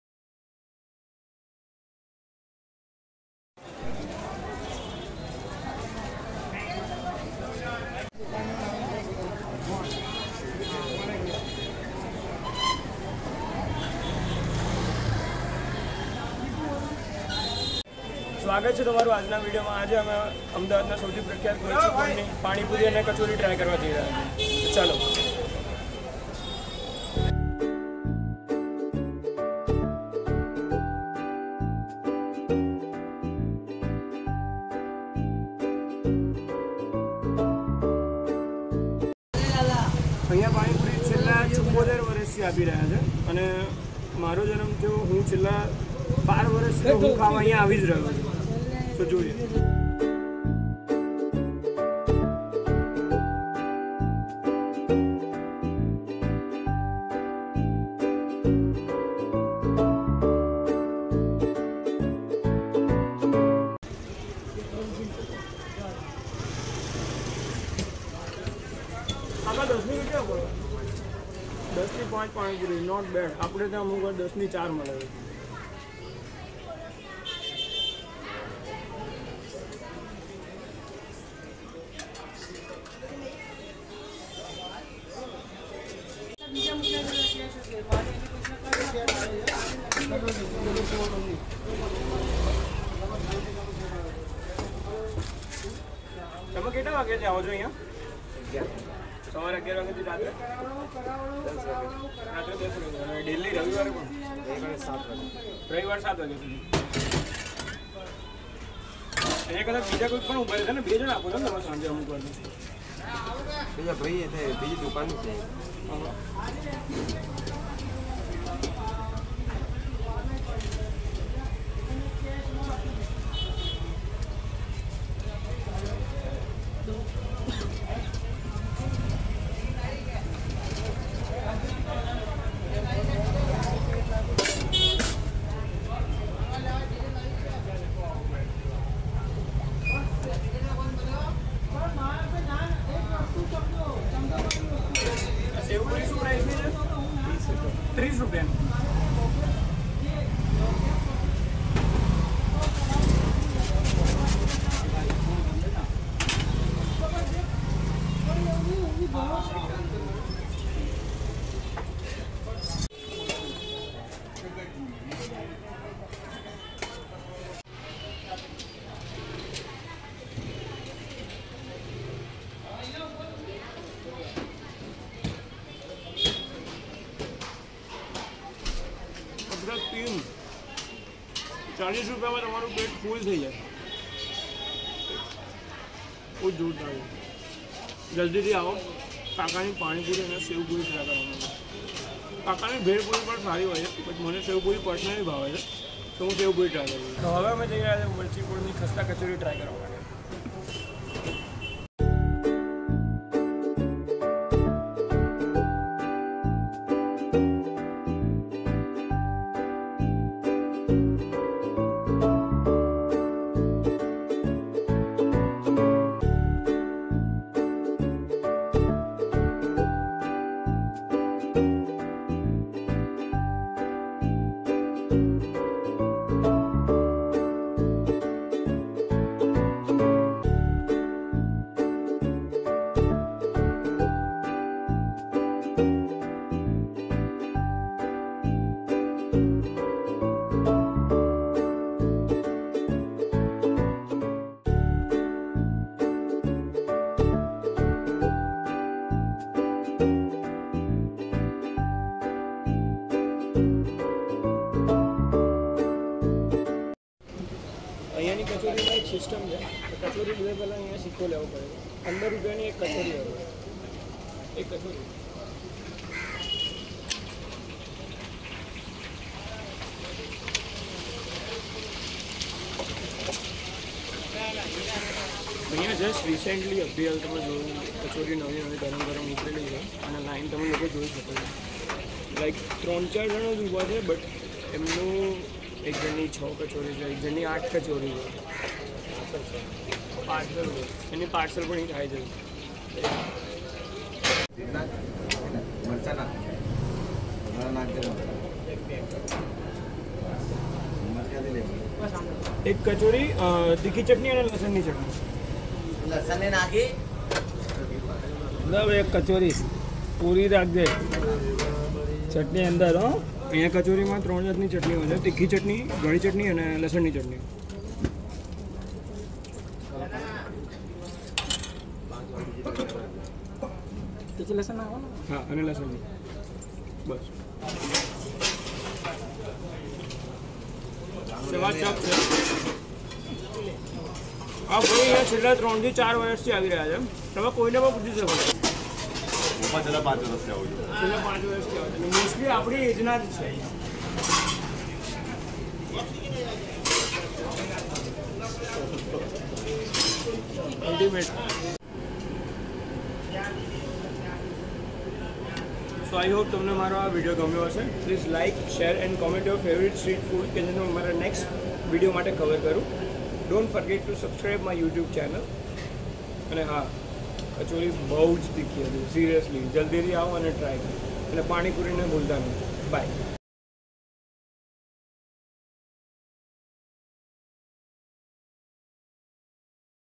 AWESOME AND BEST PANIPURI AND KHASTA KACHORI FROM MARCHI POLE | RATAN POLE | Street Food Ahmedabad
In this video, we will show you the tastiest Street food which you can try at Marchi pol - Ratan pol. The Most Popular Place For Kachori in Ahmedabad and also Panipuri and Sevpuri.

PaniPuri - Rs.10 ki 5
Sevpuri - Rs. 30/-
Khasta Kachori - Rs.15/-

Address - Marchipole, RatanPole, Ahmedabad - 380001